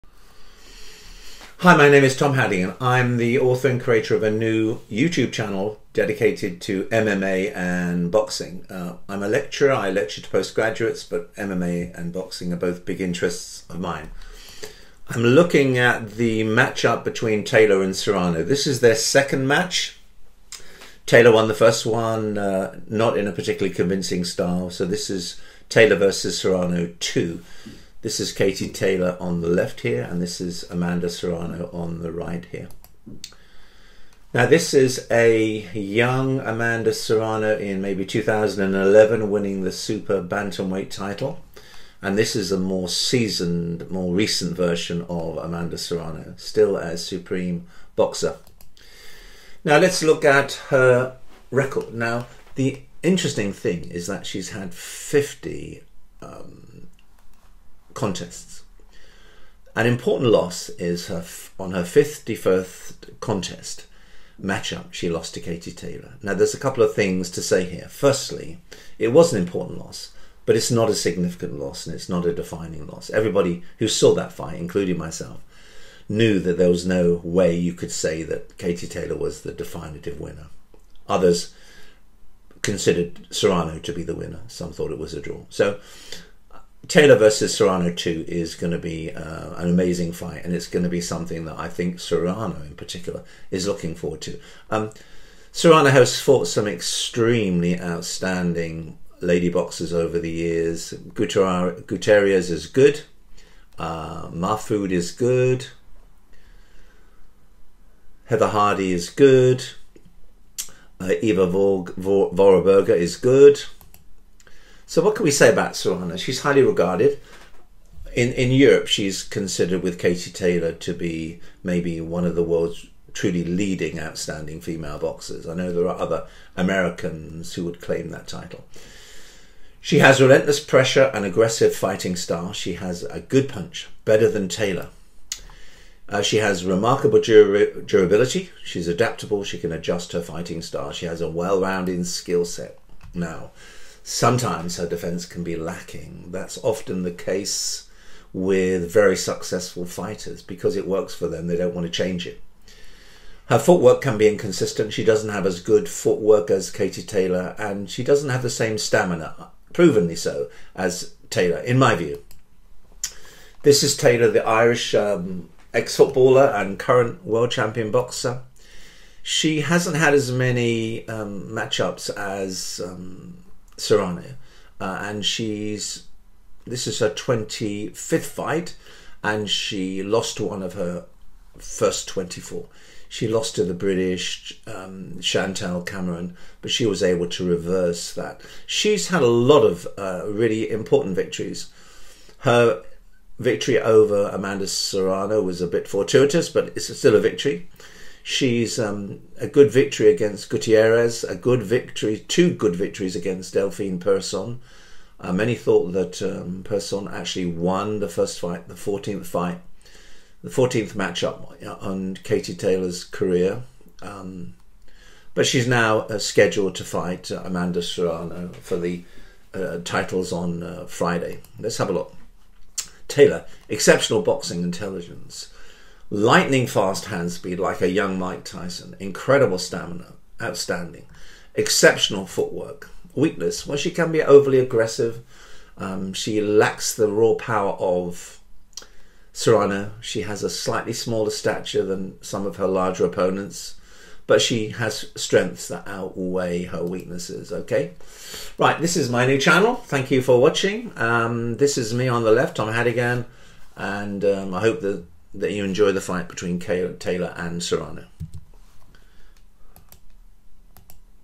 Katie Taylor v Amanda Serrano 2
The second fight between Taylor and Serrano on Friday November 15th
Katie Taylor
Amanda Serrano